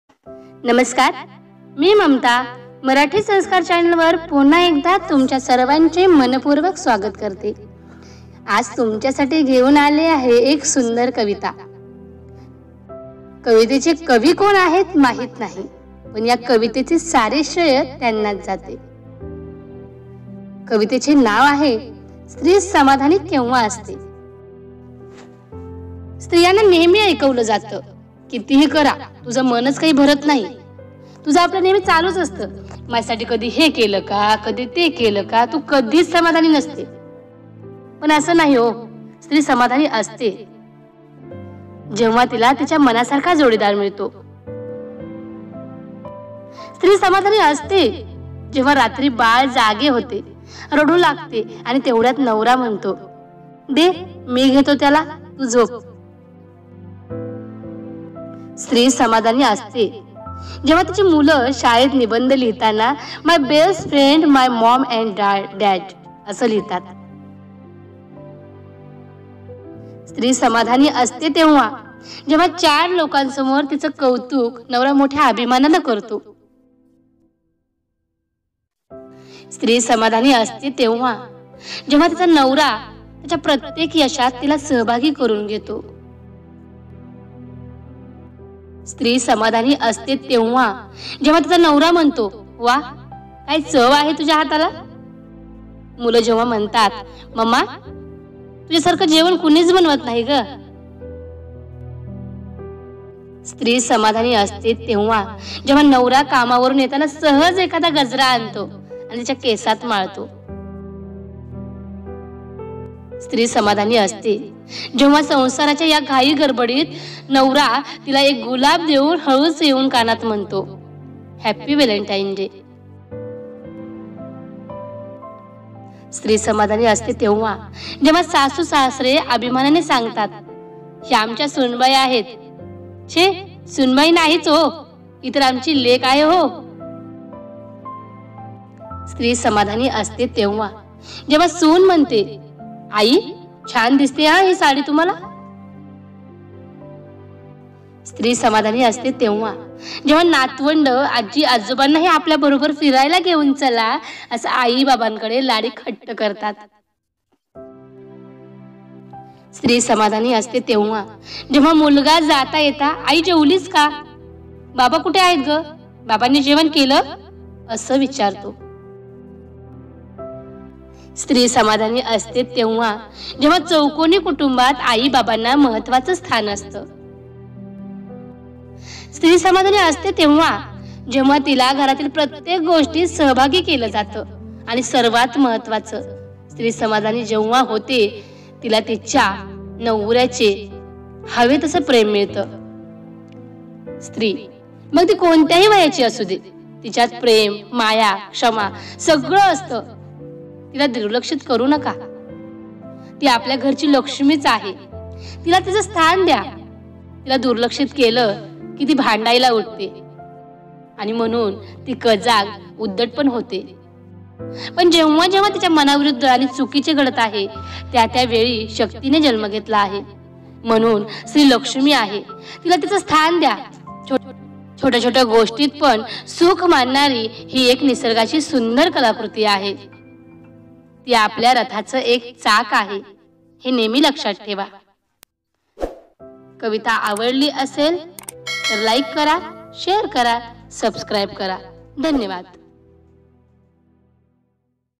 जाणून घ्या *स्त्रि समाधानी केंव्हा असते* | marathi stories with family लेखिका-सौं पुष्पा राजेश राऊत
जाणून घ्या *स्त्रि समाधानी केंव्हा असते* | marathi stories with family | सुखी जीवनाचा मूलमंत्र

लेखिका-सौं पुष्पा राजेश राऊत

सुंदर विचाराचा संग्रह- ८
मैत्री विचार